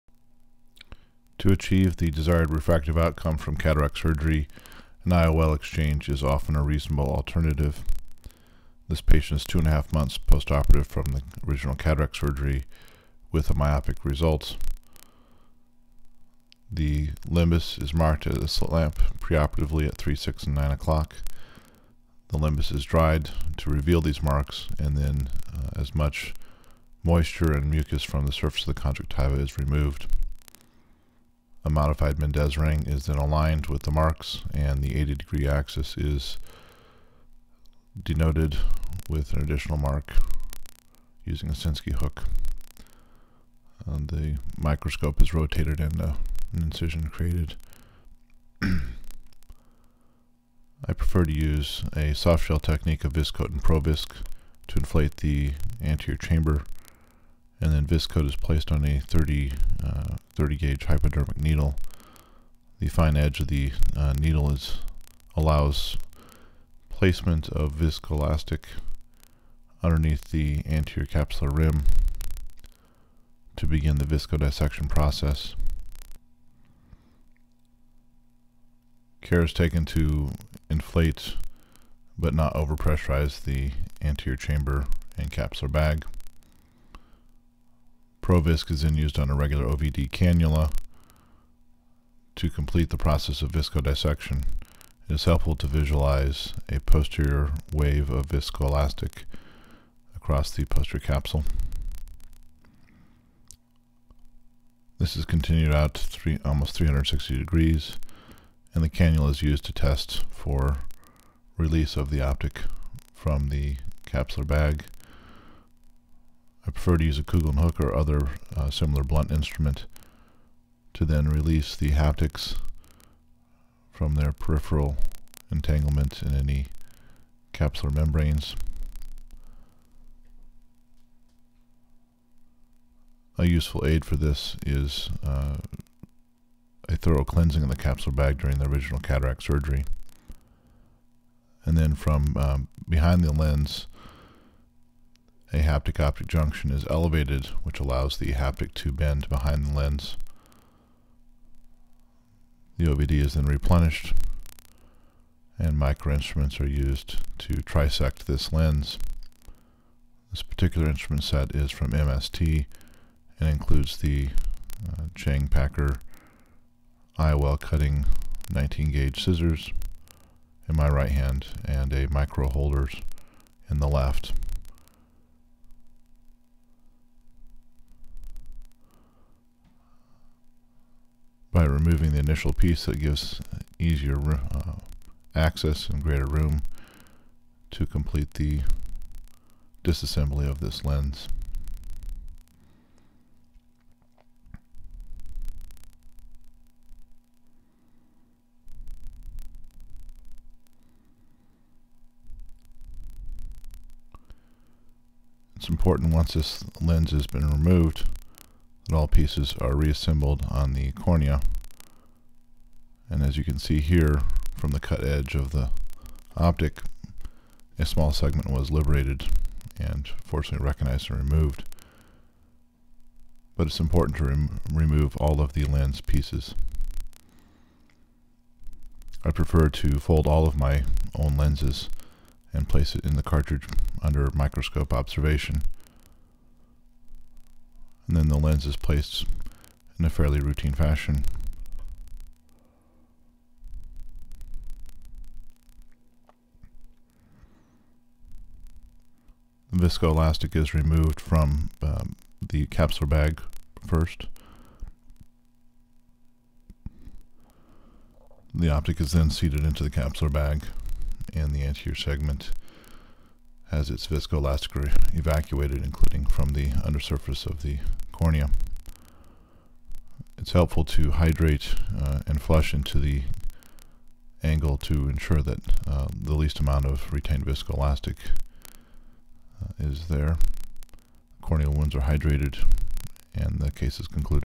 Restor to Restor IOL Exchange
To achieve the desired refractive outcome following cataract surgery, many options exist and an IOL exchange is a reasonable choice in many situations.  This patient is 2 and 1/2 months post-op from the original cataract surgery, and despite multiple pre-op measurements and calculations the refractive result was myopic.  Given the options, the patient chose IOL exchange.  Wound placement, viscodissection, trisection of the IOL and efforts to ensure complete removal of the lens are all important elements to achieving a desired result.